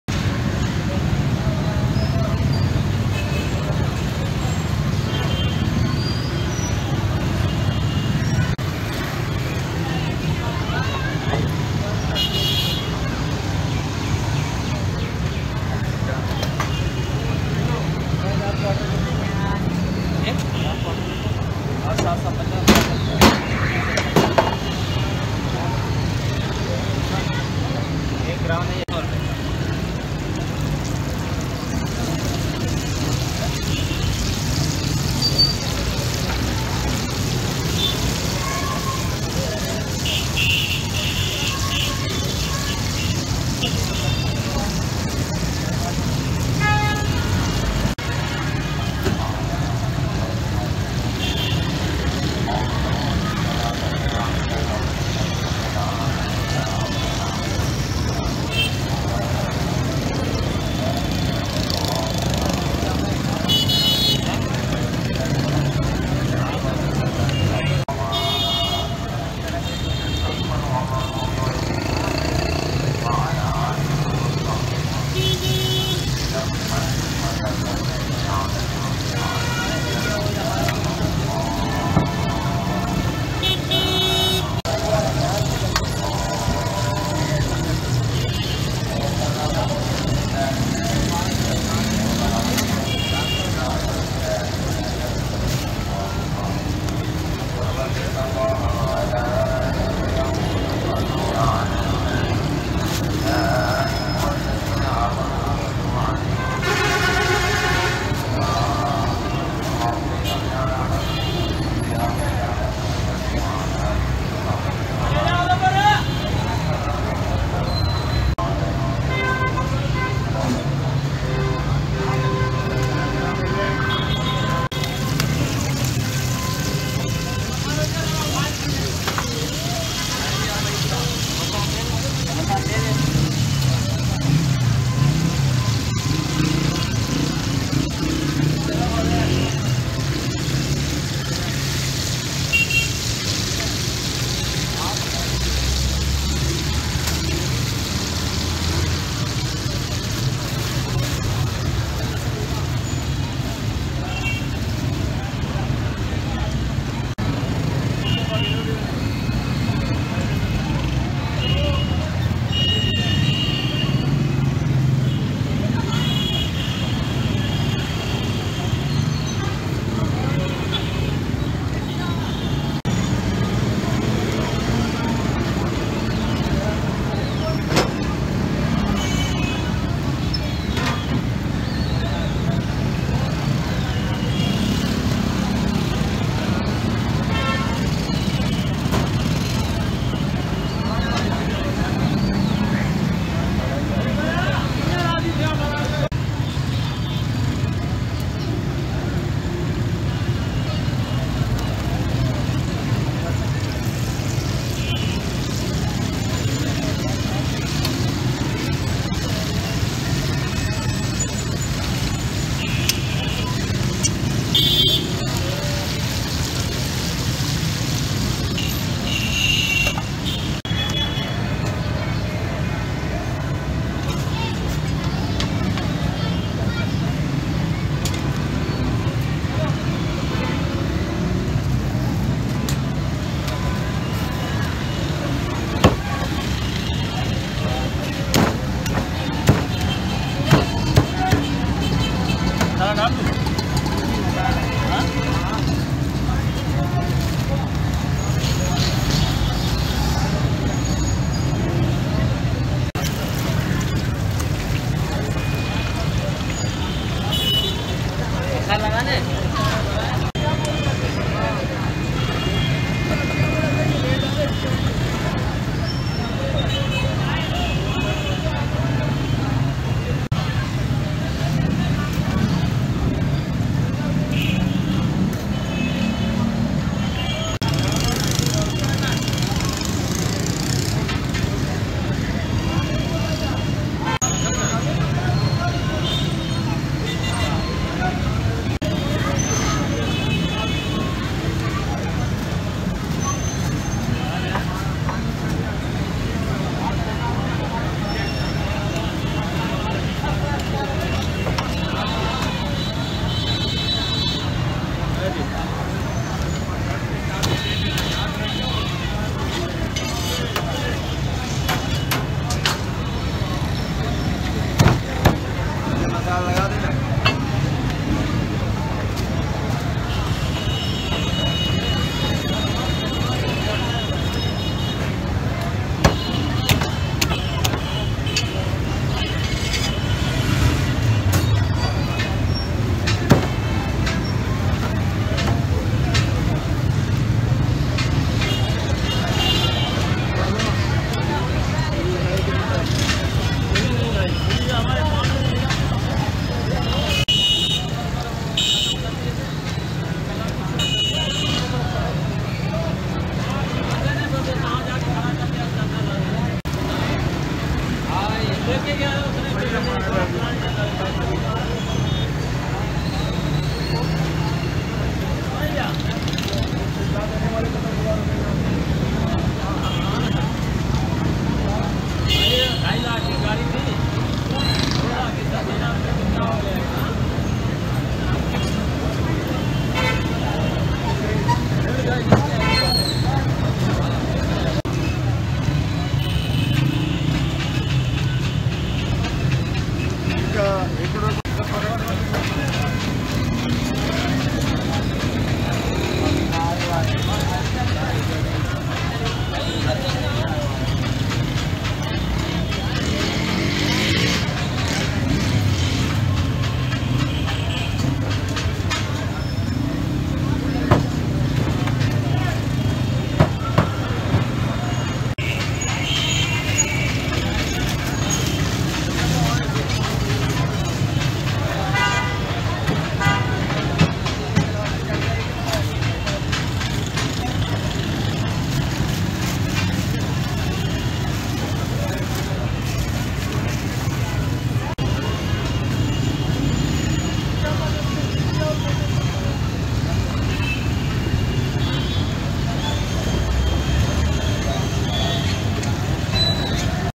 Chicken Fried and Lahori Masala Fried | Saji Street food of Pakistan | Special Karachi Chicken
Chicken Fried and Masala Saji Street food of Pakistan | Special Karachi Chicken Saji , Best Fried chicken